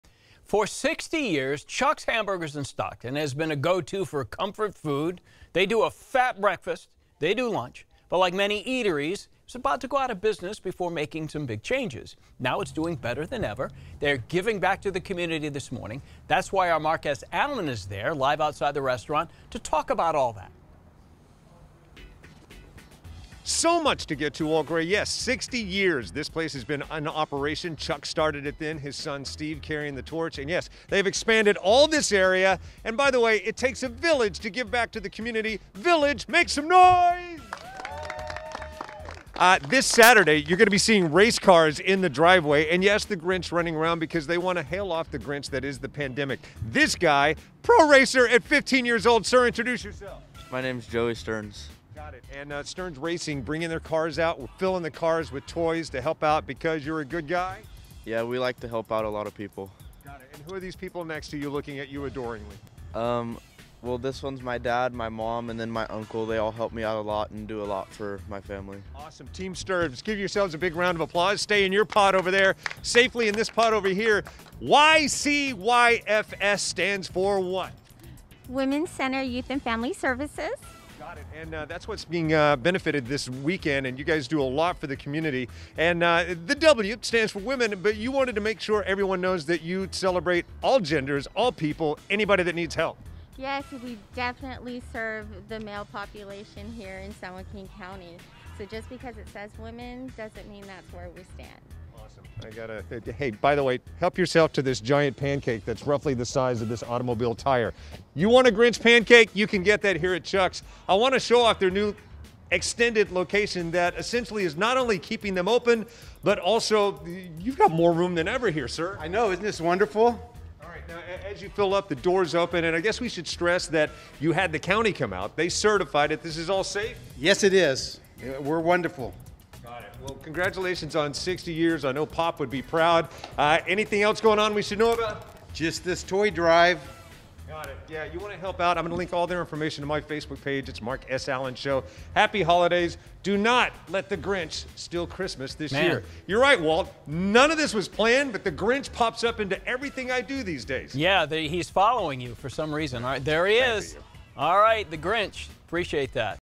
Chuck's Hamburgers in Stockton celebrates 60 years with a toy drive
The Stearns family of Stockton 99 Racing, the Women's Center Youth and Family Services, and even the Grinch are at the food spot to help.